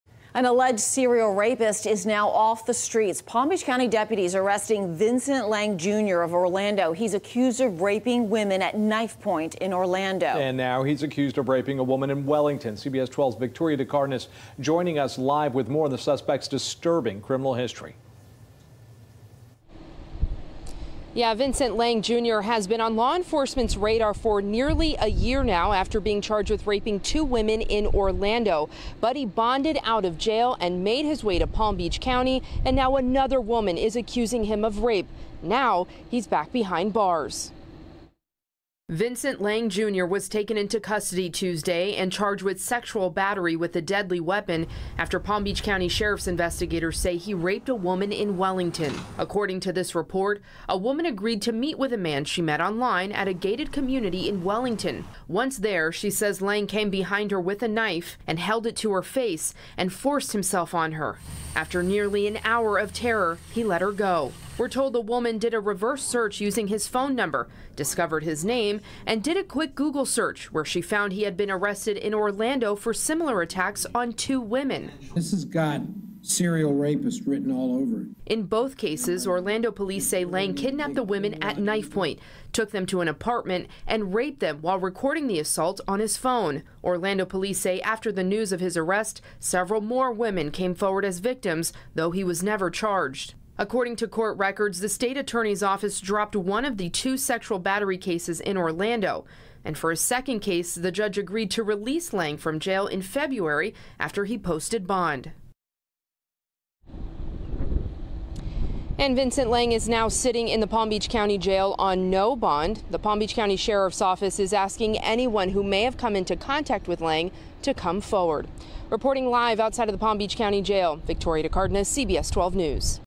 Alleged serial rapist arrested, accused of sexually battering women in Florida
A suspected serial rapist out of Orlando was arrested for sexual battery with a deadly weapon by force in Palm Beach County on Wednesday.

The Palm Beach County Sheriff's Office (PBSO) said on Tuesday, detectives from the Special Victim Unit (SVU) were looking into a case involving a 41-year-old woman who was sexually battered.

After a day, detectives were able to find probable cause to arrest the suspect, 30-year-old Vincent Ernerst Laing Jr.